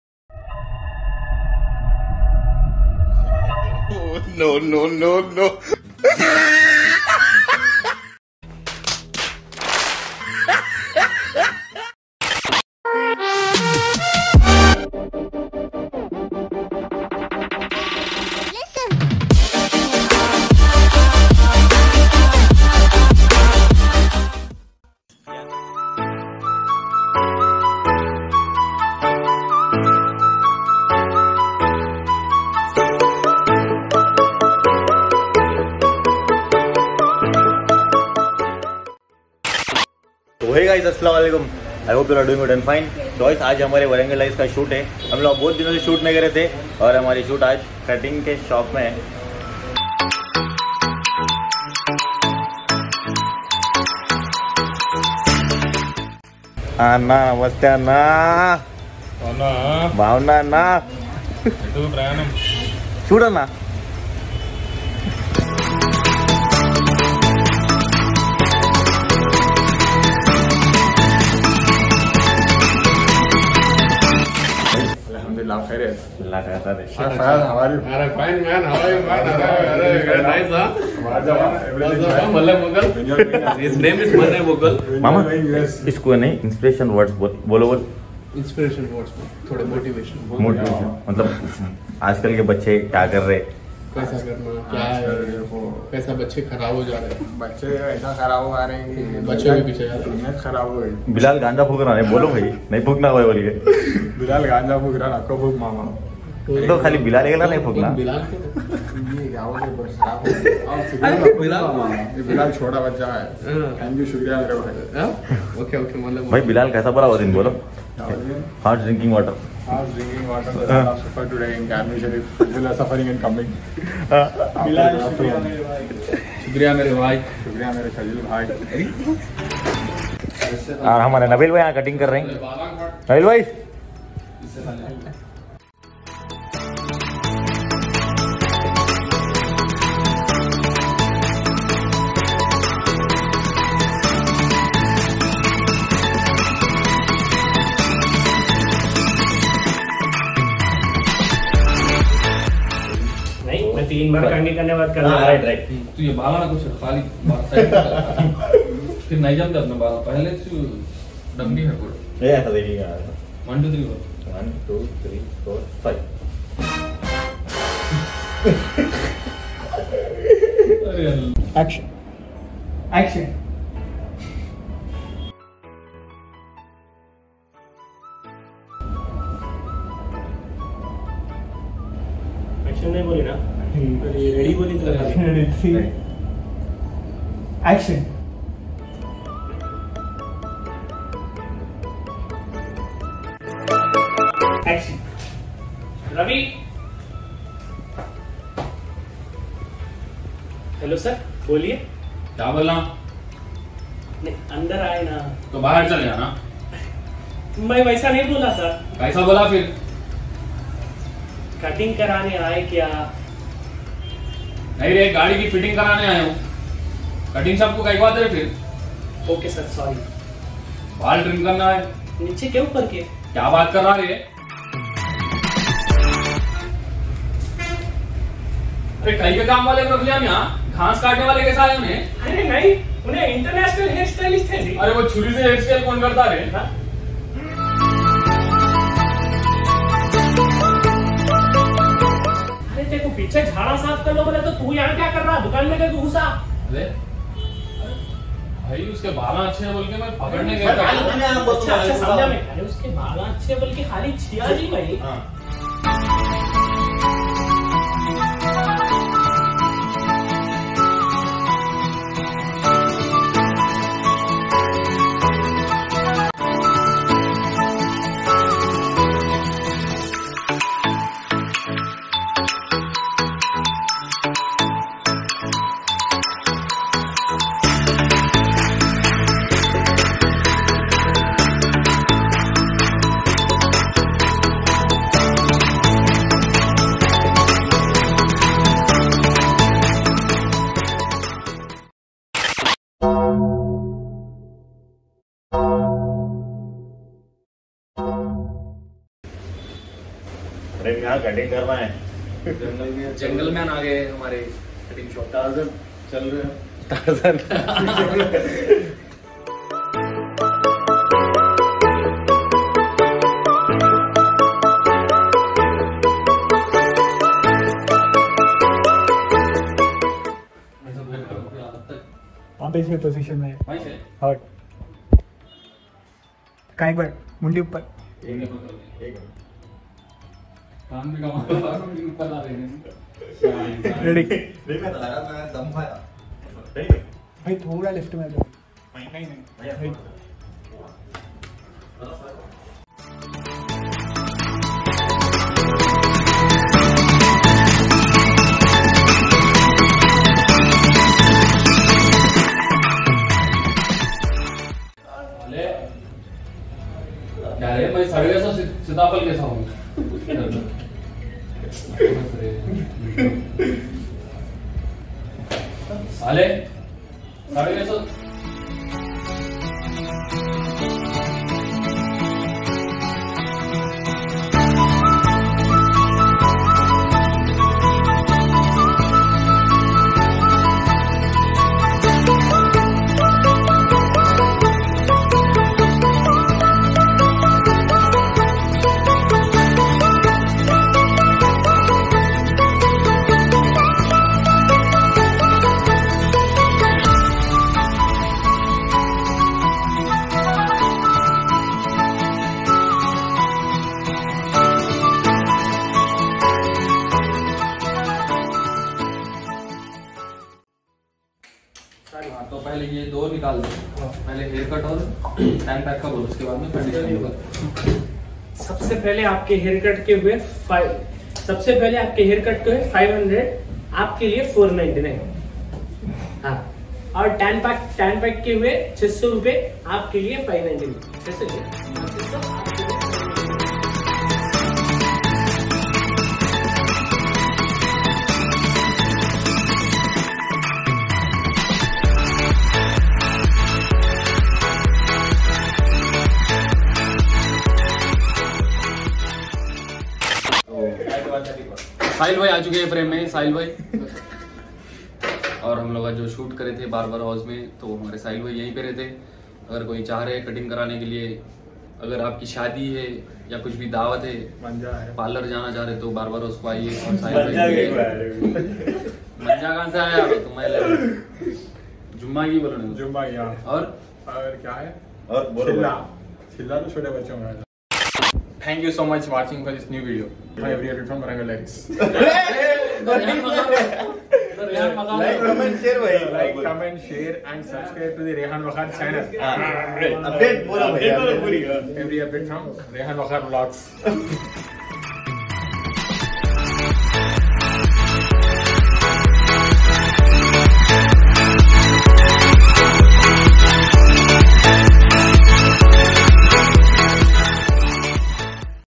How We Enjoy Shooting Our Warangal Diaries Video | The Rehaan Waqhar | Vlog 26
(Earphone Use Is Recommended)!!
Behind The Scenes Of Warangal Diaries  Which You Must Watch - Humour , Amusing , Jokes

Video Edit -  Amer Sohail